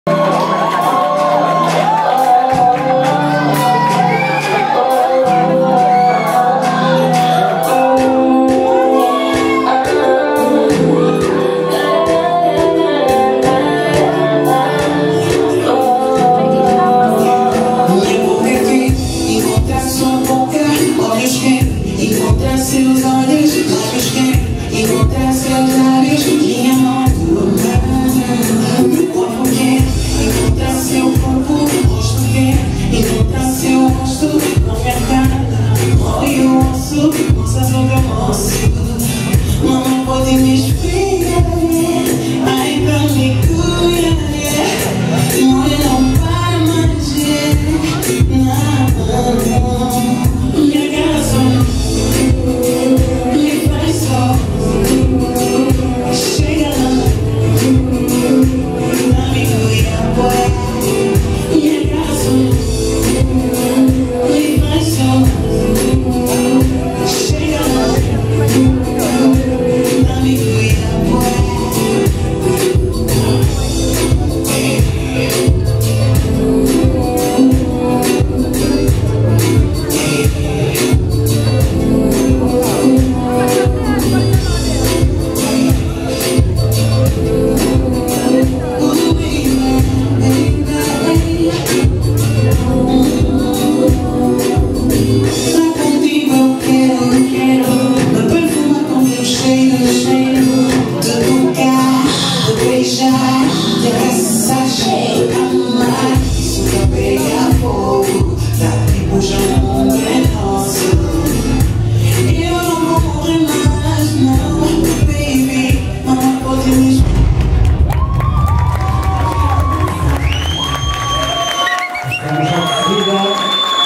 ÁfricAdançar 2015 - Javier & Belen: Spain
Javier & Belen - Couple nº 11, from Spain, in International Elimination (Improvisation Prove) of ÁfricAdançar - 8th Edition of Kizomba International Championship